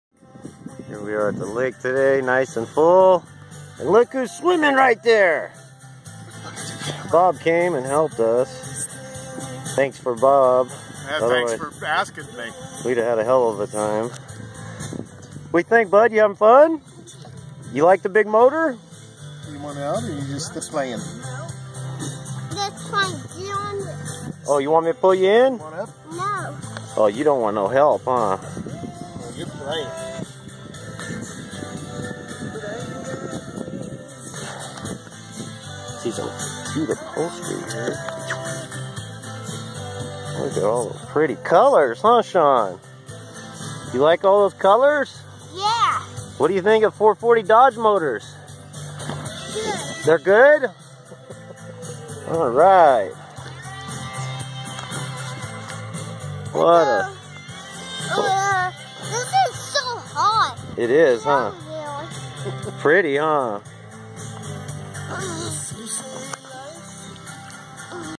Jet boat pine flat 2017
First trip taking my son in a jet boat boat hadn't been out since 2010 ran flawless